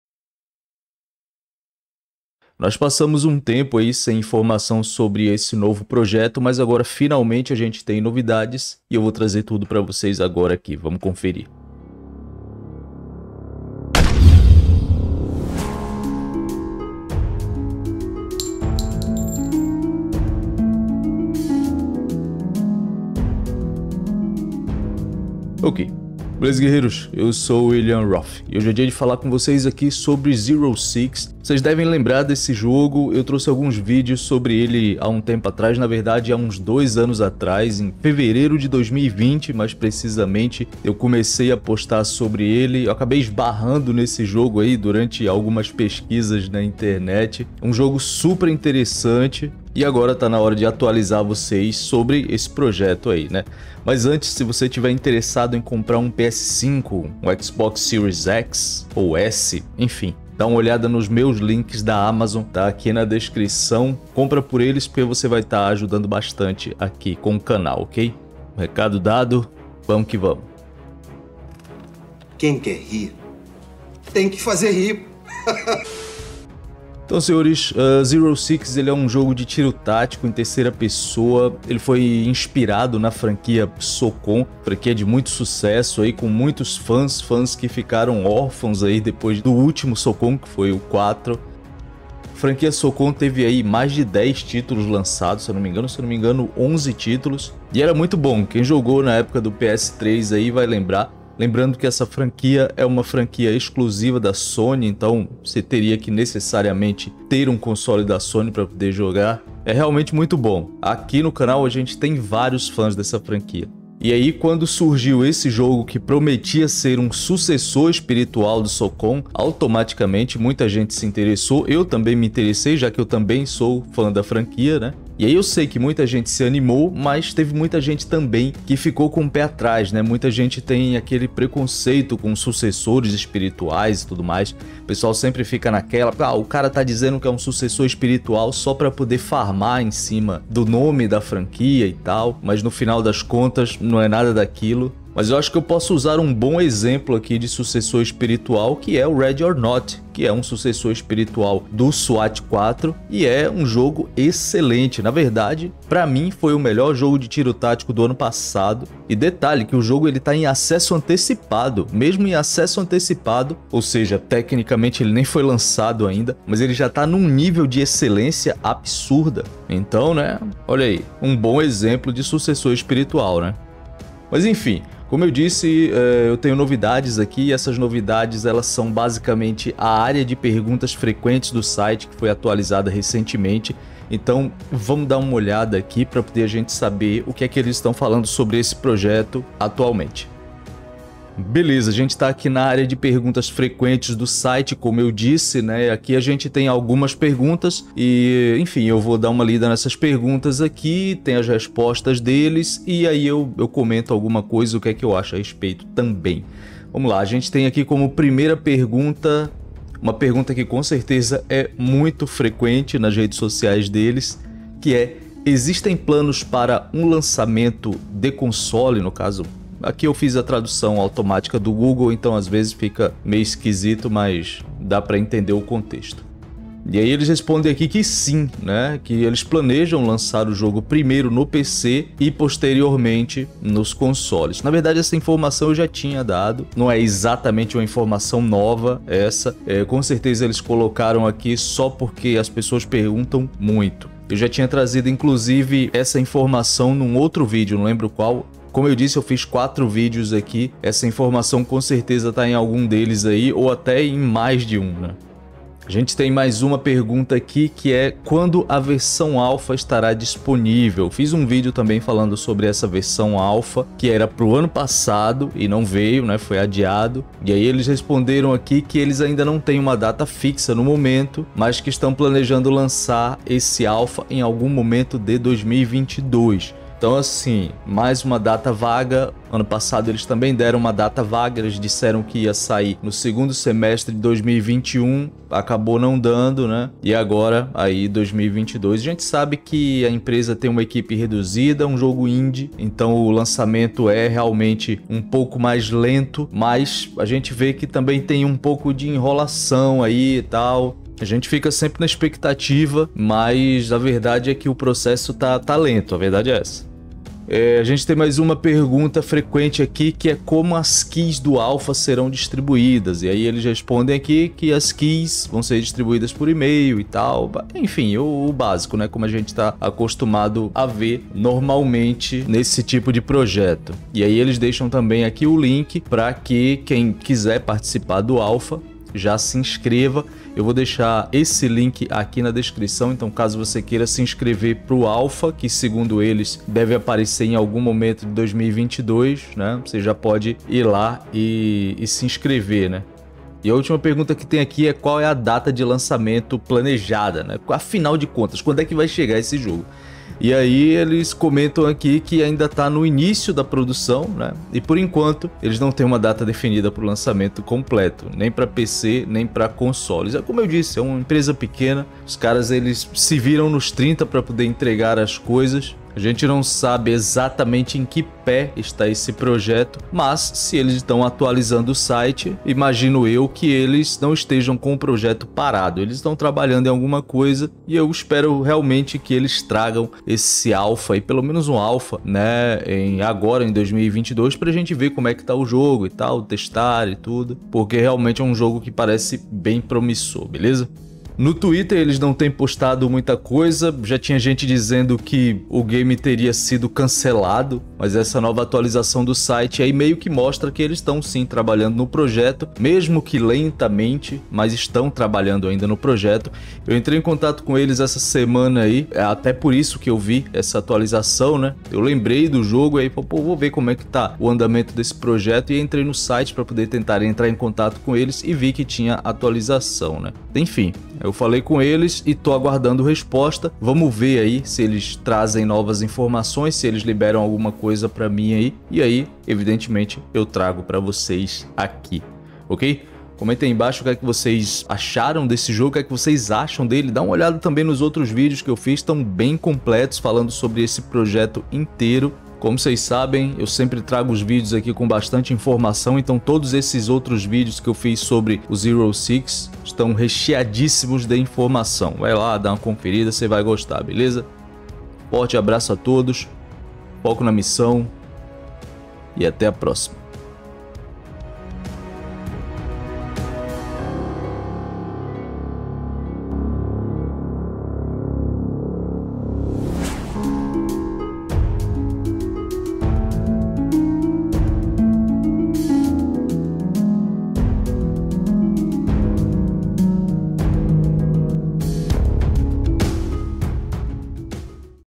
Novidades sobre o sucessor espiritual de SOCOM - Zero Six - Behind Enemy Lines 2022
COMPRE O SEU XBOX SERIES S ou X

Hoje vou falar sobre o Alfa de Zero Six - Behind Enemy Lines. Um novo jogo de tiro tático inspirado em jogos como SOCOM, H-Hour e Ghost Recon. Bora ver?

PSN: willianrof